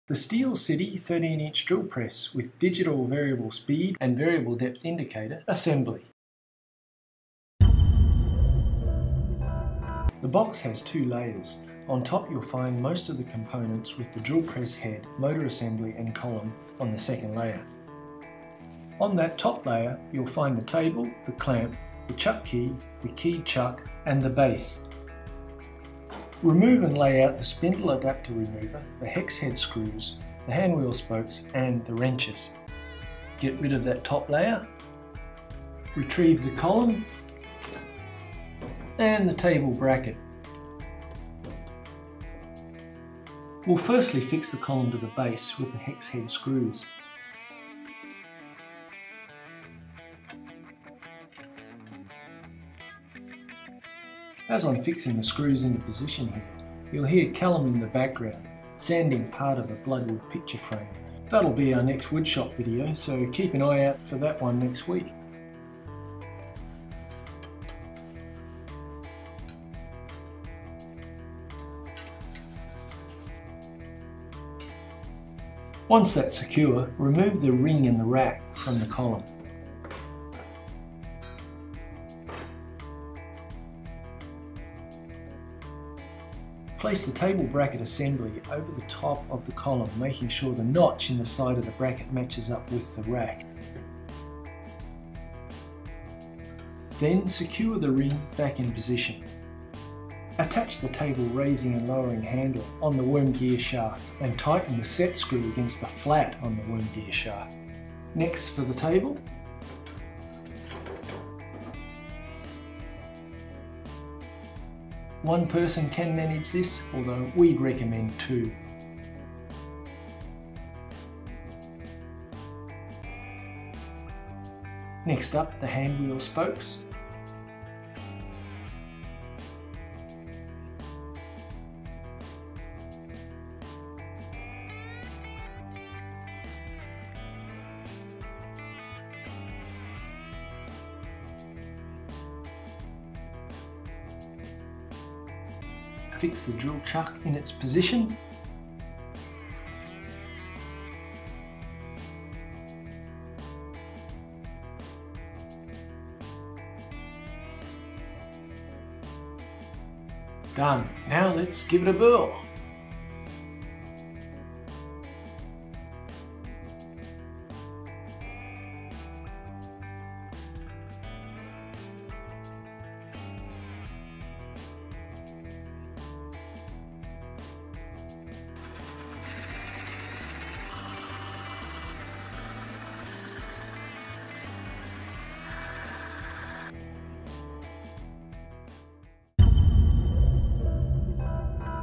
13" Benchtop Drill Press Assembly Steel City 20130VS
The steel city thirteen inch drill press with variable speed and variable depth indicator assembly

The box has two layers.

On top you'll find most of the components with the drill press head motor assembly and column on the second layer.

On that top layer you'll find the table the clamp the chuck key the keyed chuck and the base
Remove and lay out the spindle adaptor remover the hex head screws the hand wheel spokes and the wrenches. Get rid of that top layer retrieve the column and the table bracket

Firstly fix the column to the base with the hex head screws.

Once that's secure remove the ring and the rack from the column.

Place the table bracket assembly over the top of the column making sure the notch in the side of the bracket matches up with the rack.

Then secure the ring back in position
Attach the table raising and lowering handle on the worm gear shaft and tighten the set screw against the flat on the worm gear shaft
Next for the table.

One person can manage this although we'd recommend two.
Next up the hand wheel spokes.
Fix the drill chuck in its position.